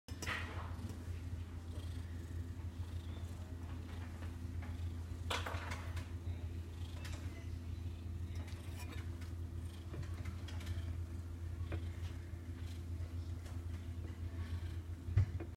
Dickie Boy at the vet hospital
Dickie Boy at the vet. He was not able to come home this day so went and spent some time with him. He was very happy to get the attention.